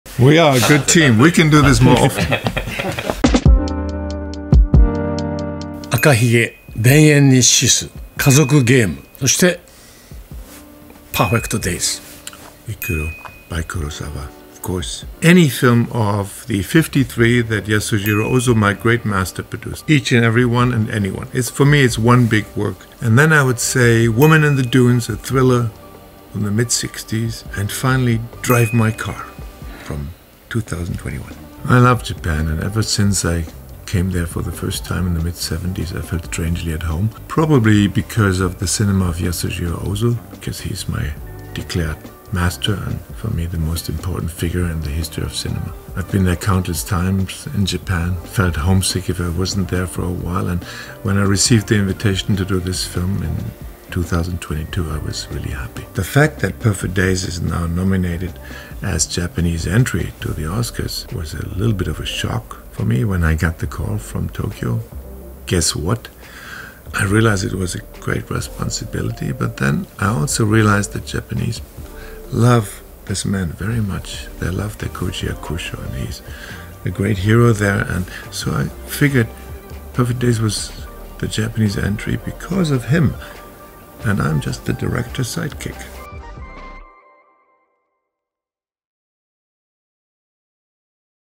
Four Favorites with Wim Wenders and Koji Yakusho
Wim Wenders and Koji Yakusho give their four favorite Japanese films and Wenders speaks to the honor of representing Japan at the 2024 Oscars.

Perfect Days lands in US theaters this February.

Interview by Brian Formo
Edited by @CultPopture
Translation by Alicia Haddick
Shot by @neonrated